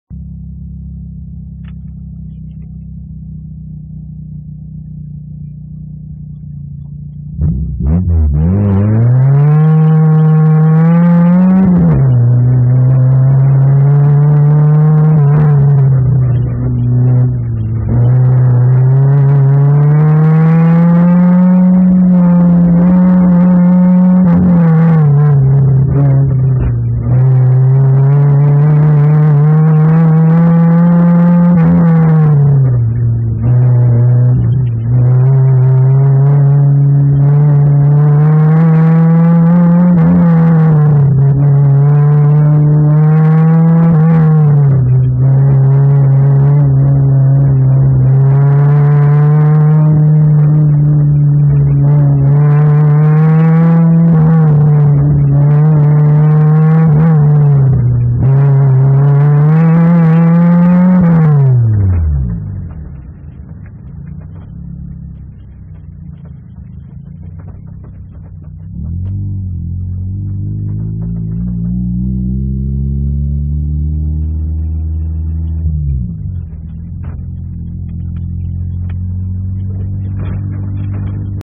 FSP #72 Ford Escort GT Furrin Autocross August 5th 2012 at MSU
1990 Ford Escort GT in FSP at MSU Lot 89 on August 5th 2012 With the Furrin Group.
Recorded on a HTC Rezound with alaprecorderhd